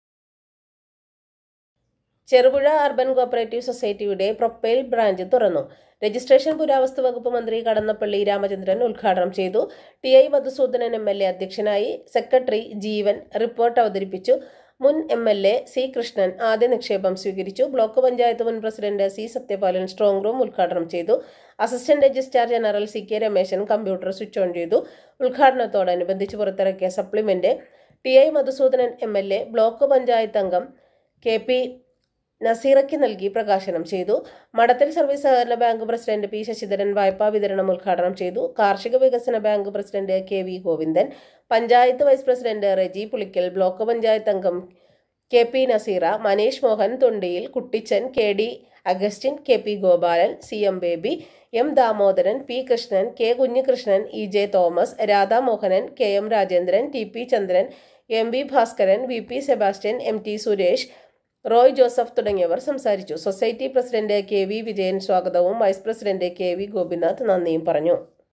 SAHAKARYAM NEWS#cooperative
ചെറുപുഴ അർബൻ സംഘം ശാഖ ഉത്ഘാടനം ചെയ്തു